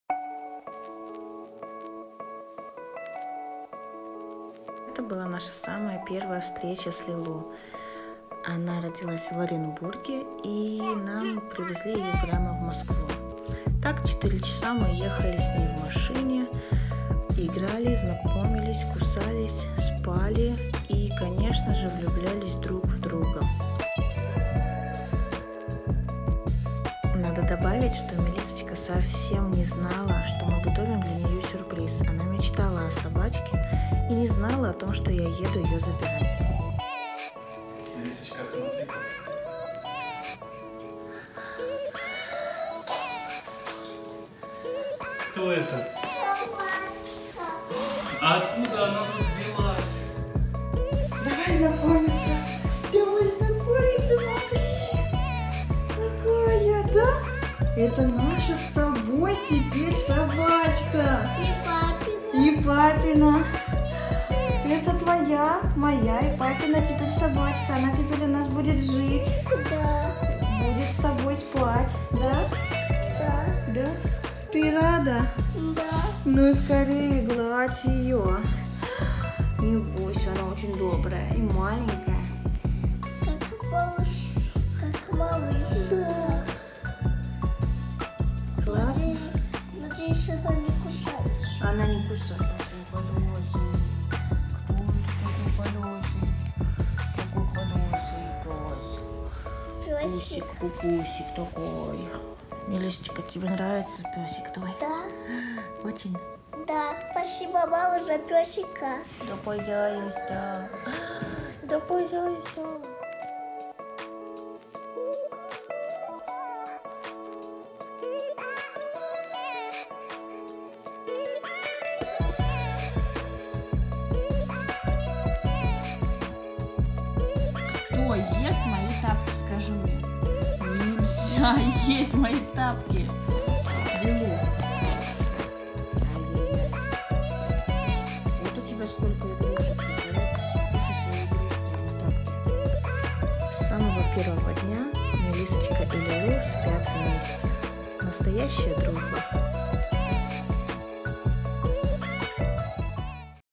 МЫ КУПИЛИ СОБАКУ| Первый день дома| Кавалер Кинг Чарльз Спаниель
Сбылась мечта и в нашем доме теперь маленький щенок! Мелисса не знала о том, что её ждёт и зачем мама уехала на день в Москву! Смотрите на наш первый день с щенком дома!
Пишите, что вам интересно узнать о нашем новом питомце?!
Радости и трудности жизни с щенком